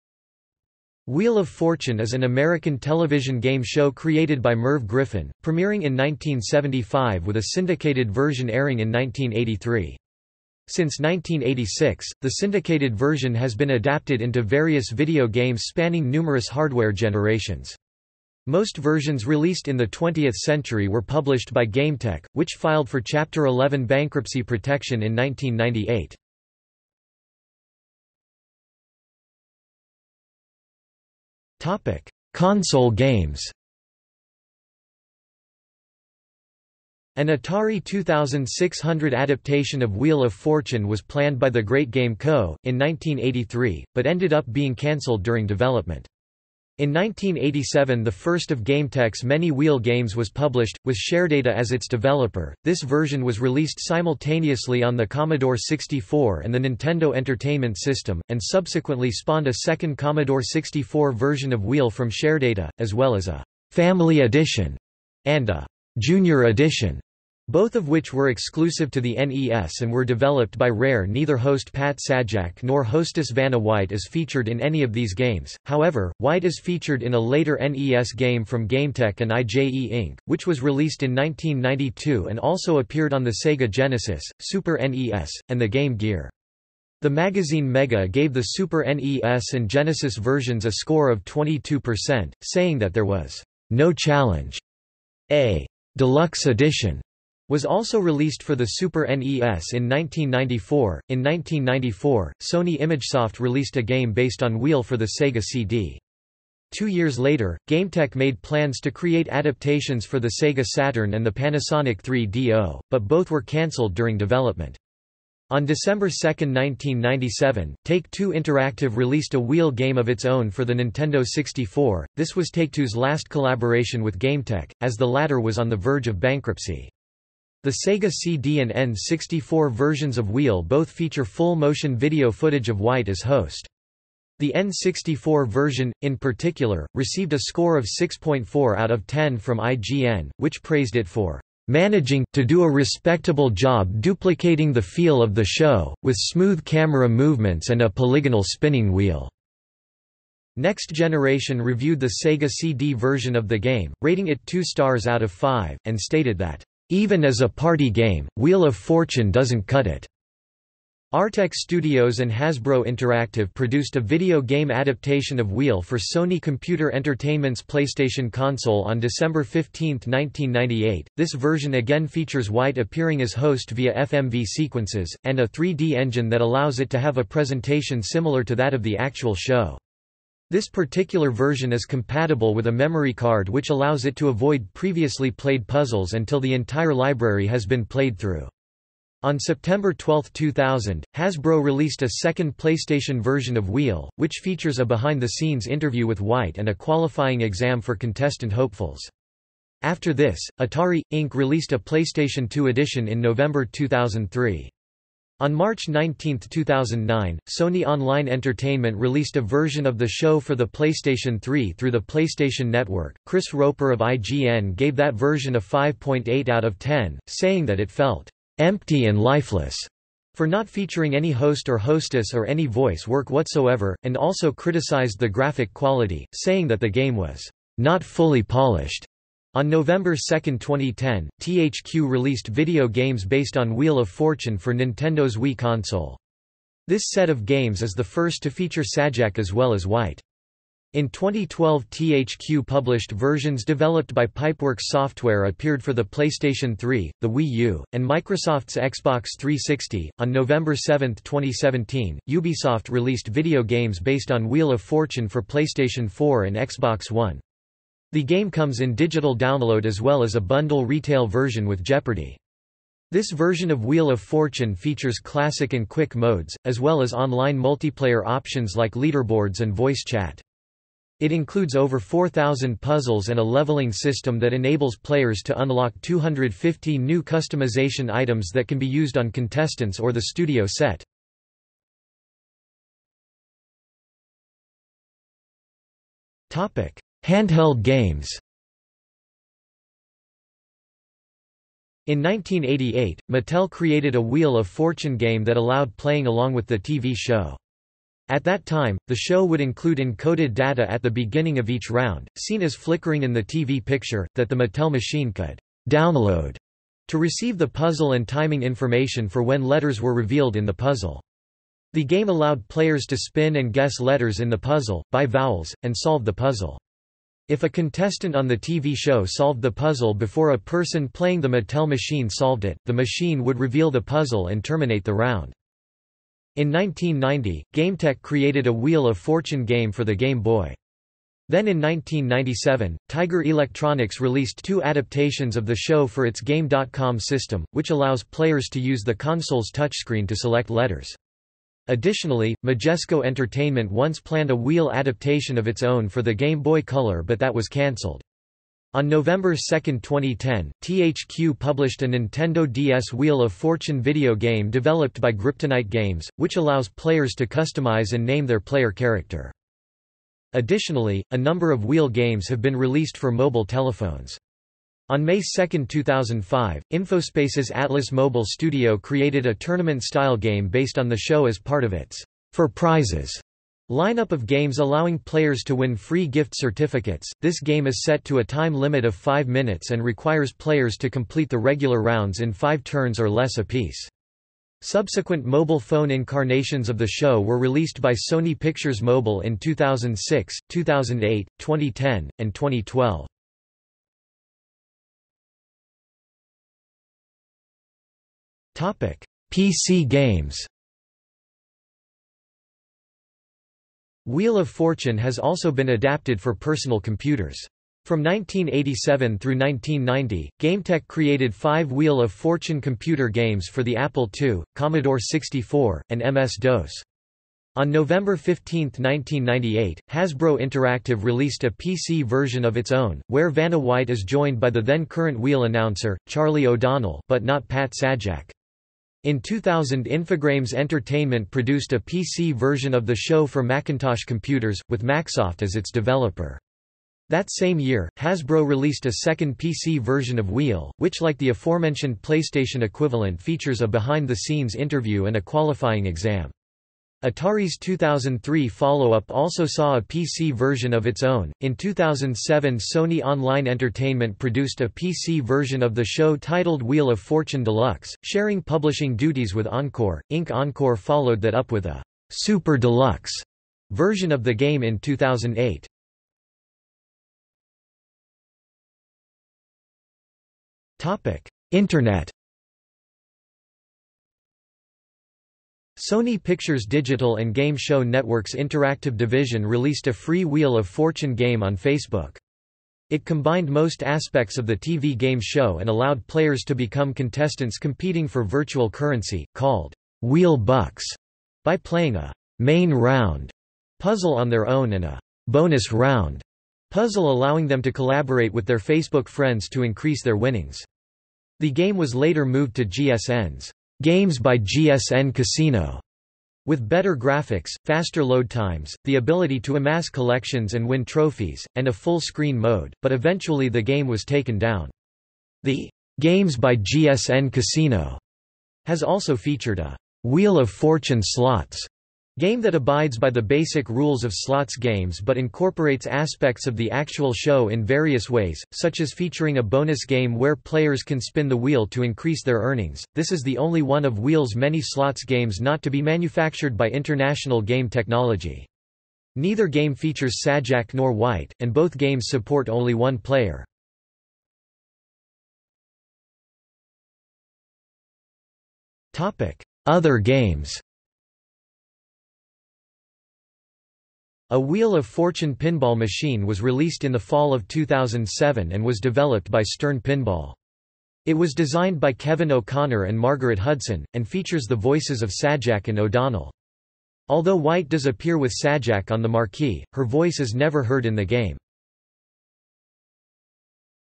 Wheel of Fortune video games | Wikipedia audio article
00:00:28 1 Console games
00:04:42 2 Handheld games
00:06:35 3 PC games
00:07:47 4 Internet
00:09:05 5 Other games

- Socrates

SUMMARY
Wheel of Fortune is an American television game show created by Merv Griffin, premiering in 1975 with a syndicated version airing in 1983. Since 1986, the syndicated version has been adapted into various video games spanning numerous hardware generations. Most versions released in the 20th century were published by GameTek, which filed for Chapter 11 bankruptcy protection in 1998.